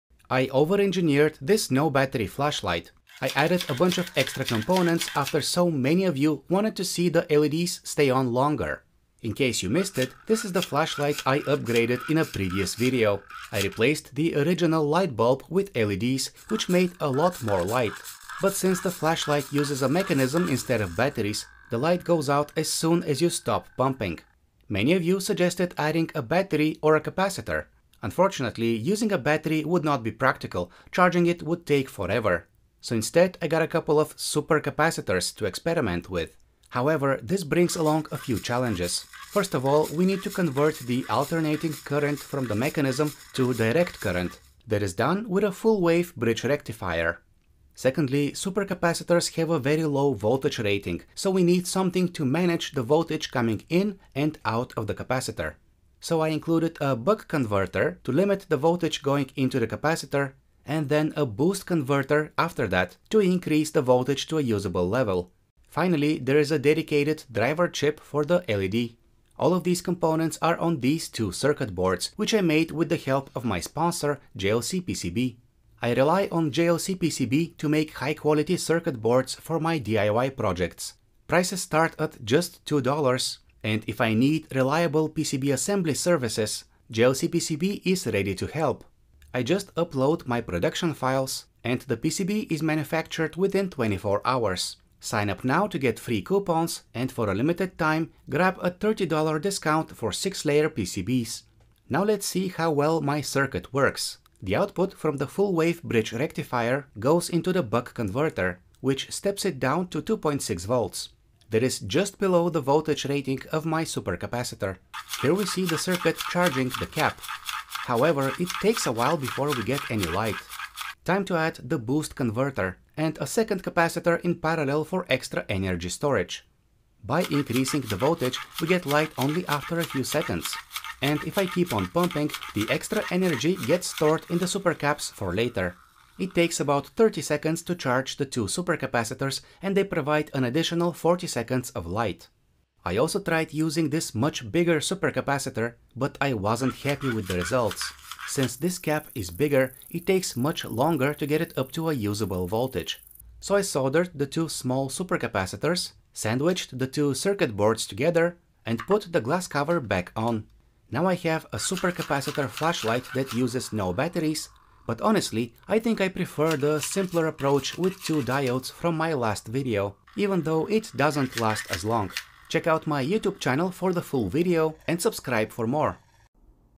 I added a supercapacitor to the no-battery flashlight
Discover Easy, Affordable, and Reliable PCB manufacturing with JLCPCB!
*Special Deal:* Get a $30 coupon for JLCPCB premium 6-layer PCBs

I added a supercapacitor to the no-battery flashlight. I also added buck and boost converters to manage the voltage coming in and out of the capacitor. This allows the flashlight to stay on even after I stop pumping the handle for the mechanical generator.

I ended up using two 2.7V supercapacitors which provide an additional 40 seconds of light when fully charged. The full build video is on my YouTube channel.